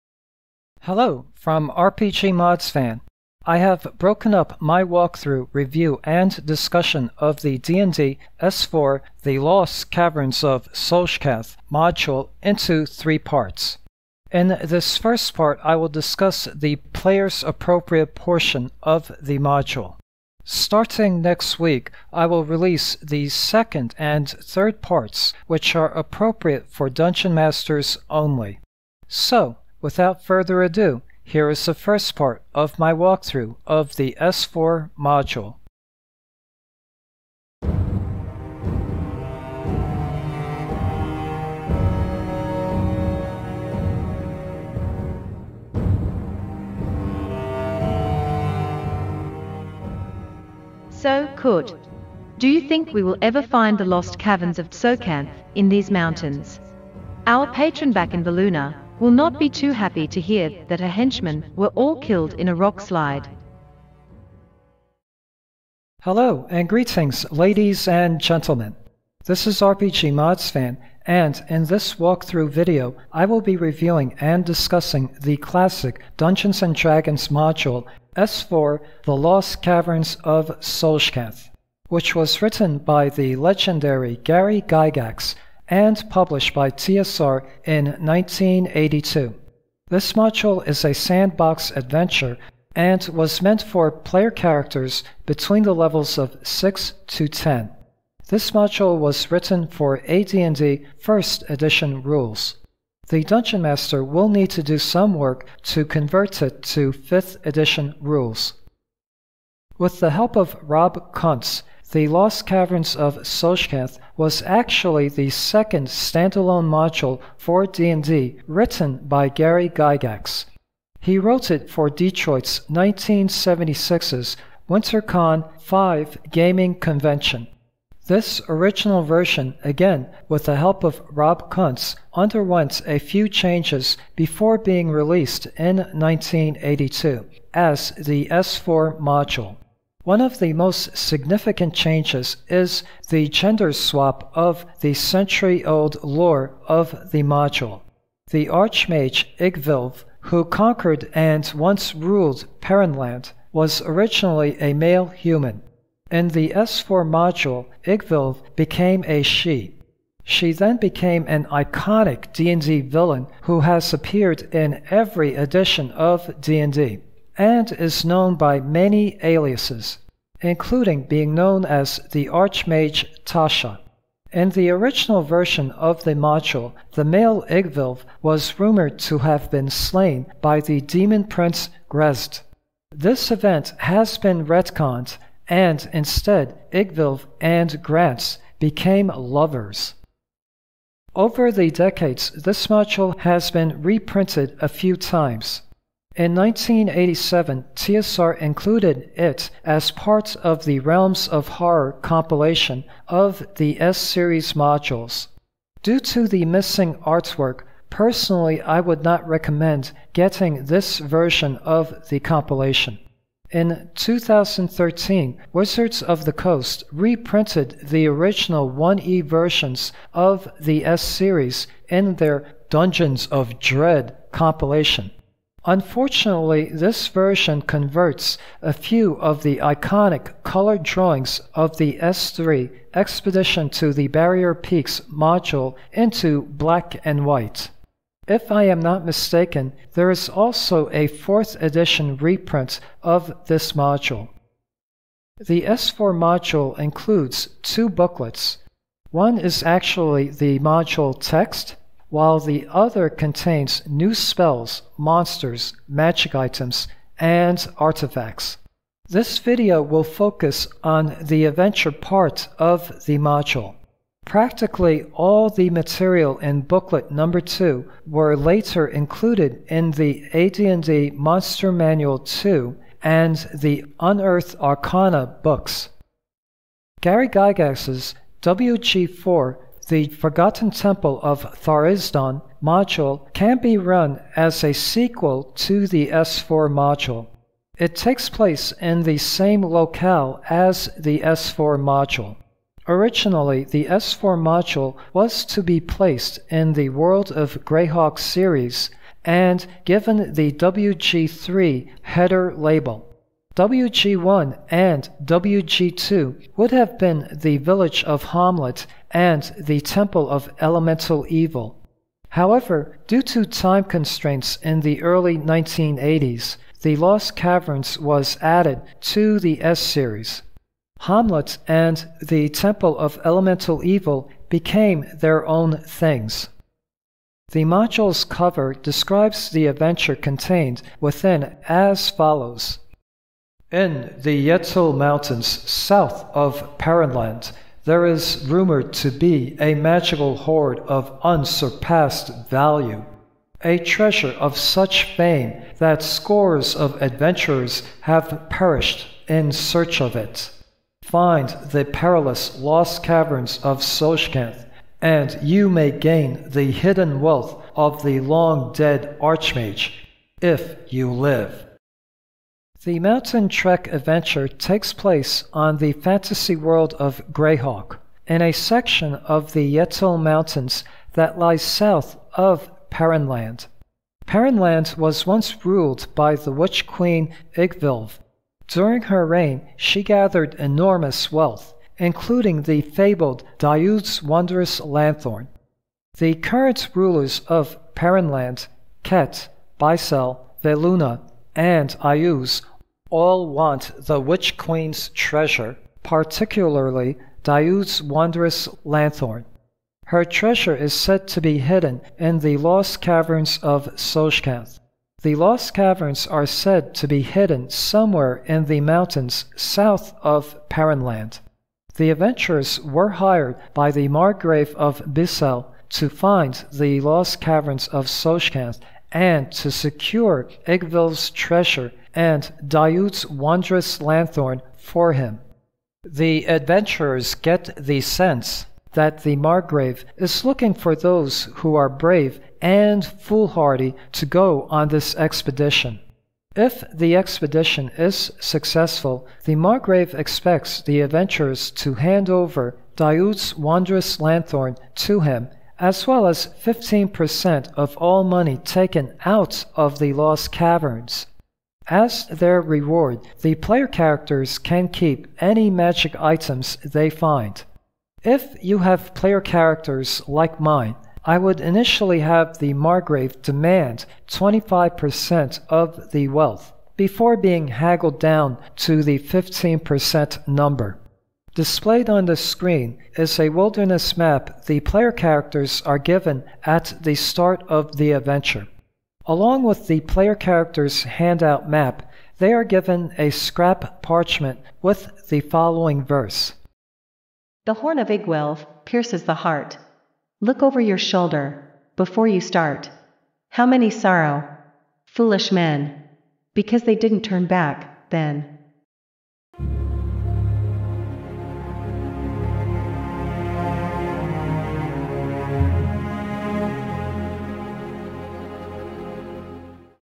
(Part 1 of 3) S4 Lost Caverns of Tsojcanth [ RPGmodsFan ]
Part 1 (of 3) of my review of the classic D&D module:
S4 "The Lost Caverns of Tsojcanth"

I have broken up my review and discussion of the Dungeons & Dragons ""The Lost Caverns of Tsojcanth"" Module into 3 parts.
In this 1st Part, I will discuss the Player appropriate portion of the Module.

Starting next week, I will release the 2nd and 3rd parts, which are appropriate for Dungeon Masters only.

Written by the legendary Gary Gygax, and published by TSR in 1982.

Cheers,
RPGmodsFan